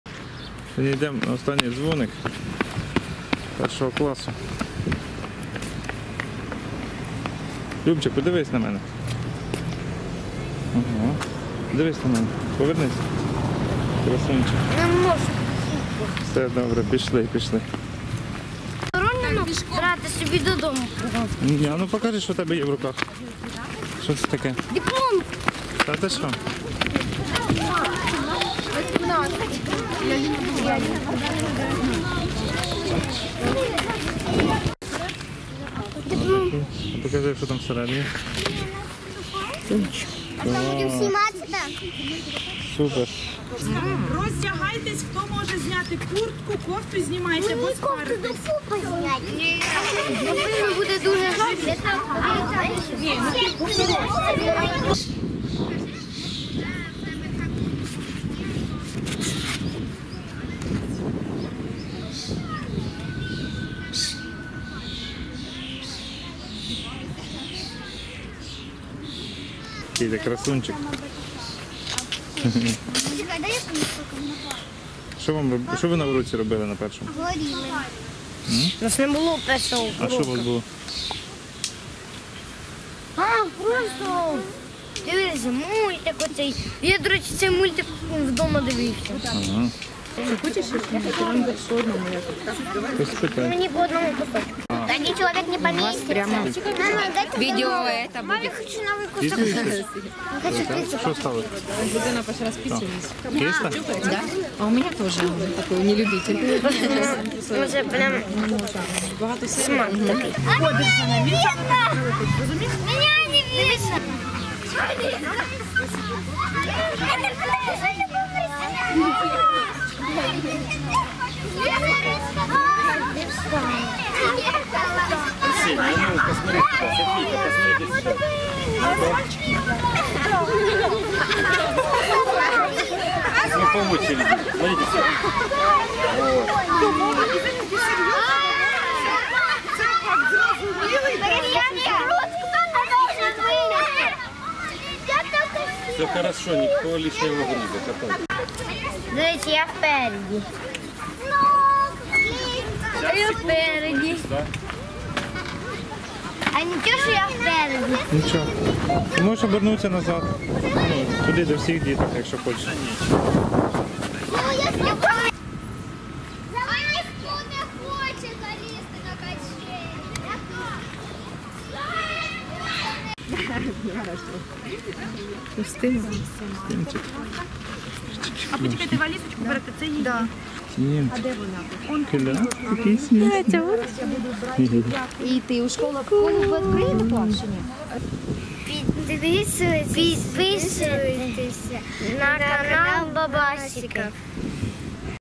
VLOG Любомир закінчив 1 клас: останній дзвоник і прогулянка | ВЛОГ Последний звонок | Babasiky
Влог про те, як найстарший Бабасік Любомир закінчив навчання в 1 класі школи. Так званий останній дзвоник і невеличкі гулянні діток у парку.
Влог о том, как самый старший Бабасик Любомир закончил обучение в 1 классе школы. Так называемый последний звонок и прогулка в парке после него.